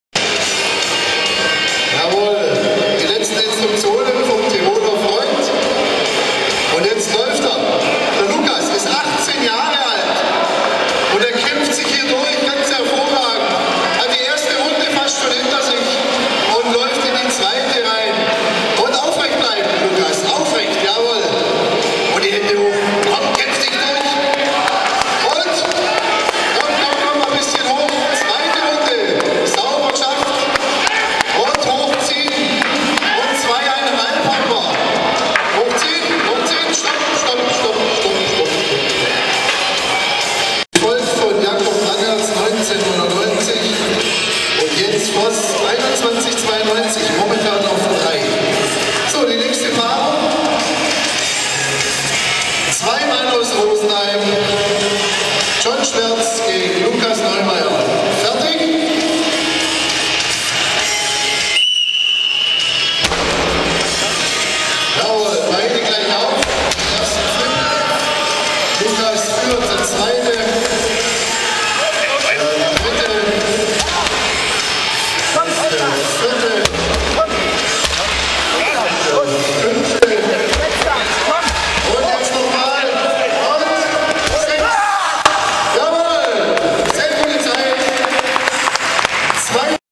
Newcomercup Schweiz 15.8.09
Mein erster richtiger Strongmanwettkampf, bei dem ich 5ter von 10 wurde.
Die erste Disziplin war der Loglift indem ich mit 105kg auf Rang 8 kam. In der zweiten Disziplin, dem Farmerswalk wurde belegte ich den 2. Platz . Es war das erste mal dass ich das Conanswheel gemacht habe. Hier kam ich auf Rang 3. Die letzte Disziplin war der Wheelflip mit 300kg. Hier bin ich links im Video im Duell mit meinem Freund John Schwertz. Ich wurde beim Wheelflip 5ter, er 6ter.